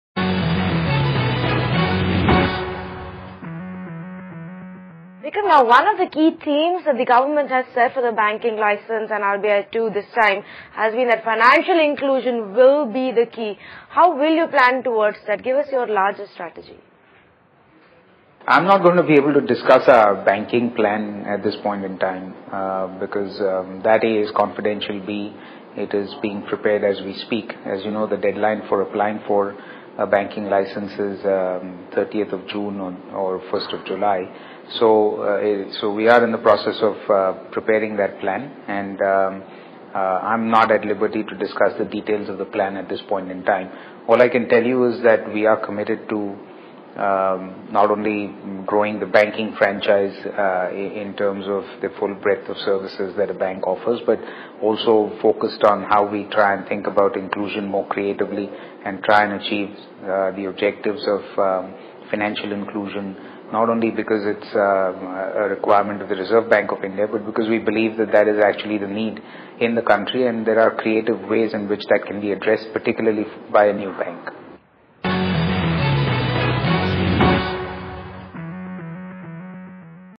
In Business- IDFC On Financial Inclusion
"We are not discussing banking plan as of now. But we are in the process of preparing banking plan," said Vikram Limaye, MD & CEO, IDFC.